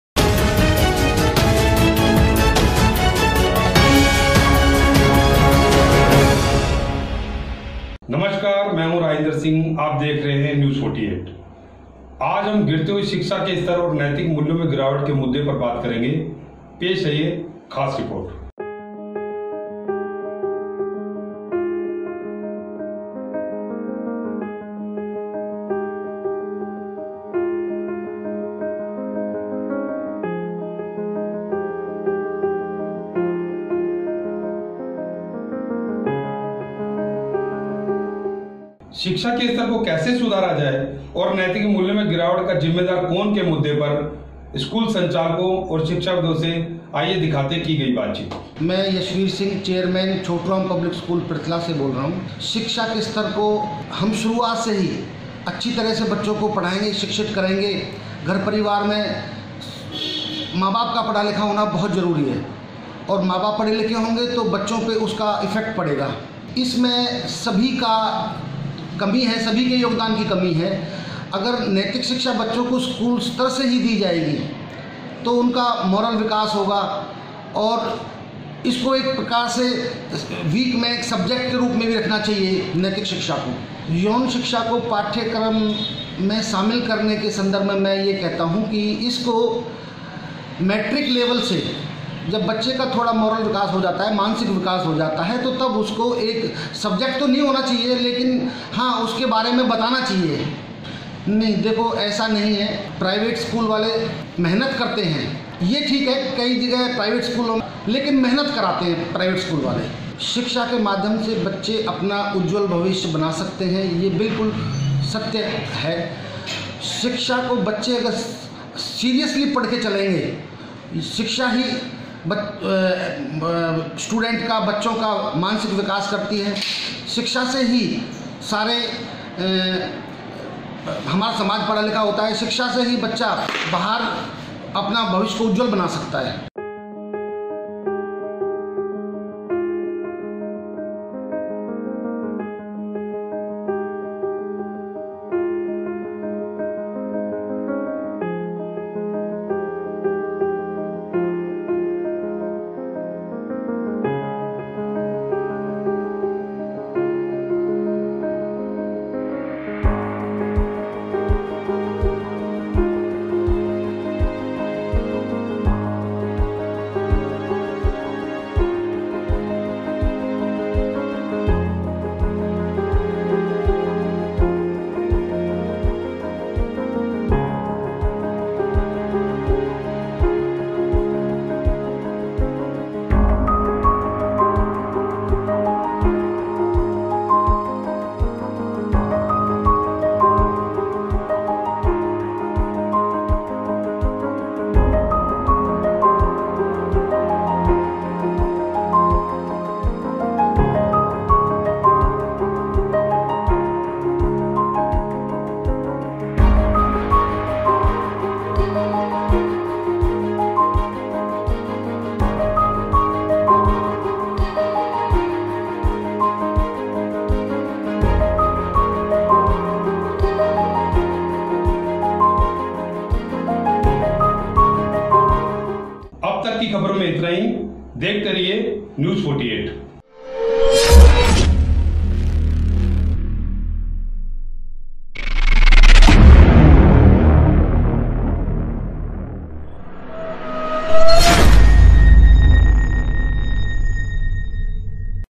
पलवल | नैतिक शिक्षा हो पाठ्यक्रम में शामिल | C.R Public School
शिक्षा में सुधार के लिए आवश्यक है शिक्षित अभिभावक शिक्षित अभिभावकों से बच्चो के पढ़ाई मंं मिलेगा सहयोग मूल्यों में गिरावट के जिम्मेदार शिक्षक,समाज,अभिभावक